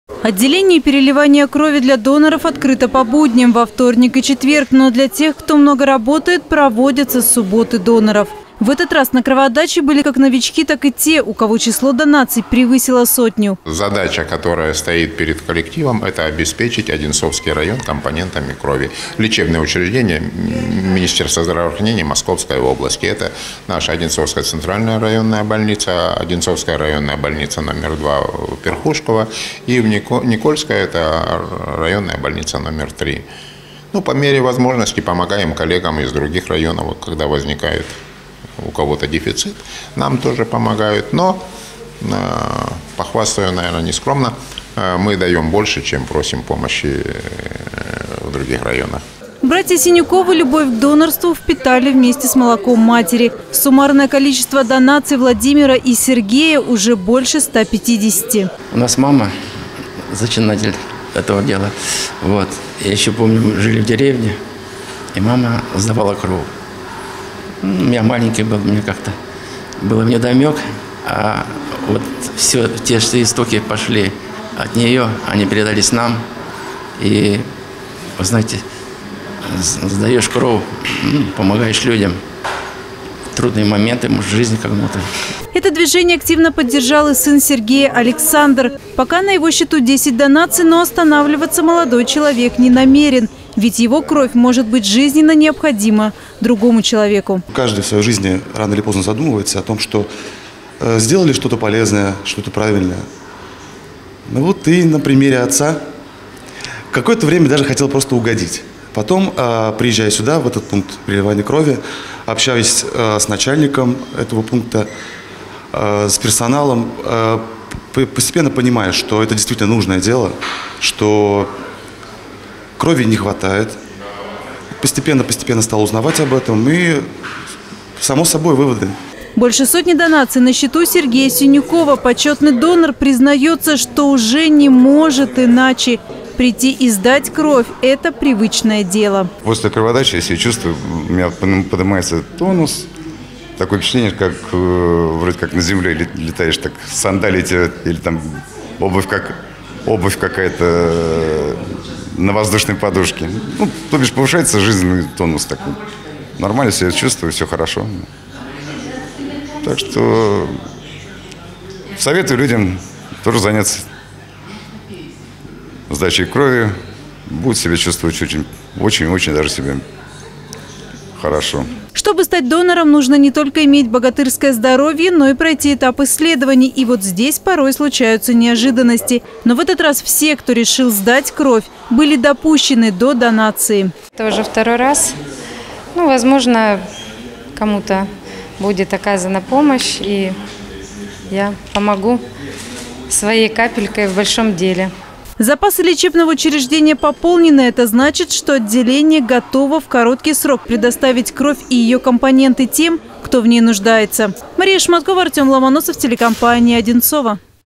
День донора в отделении переливания крови
В период летних отпусков отделение переливания крови Одинцовской ЦРБ испытывает дефицит в ценных компонентах, именно поэтому каждый донор сейчас – на вес золота.  Удалось ли медикам наполнить хранилища плазмой крови по итогам прошедшей донорской акции?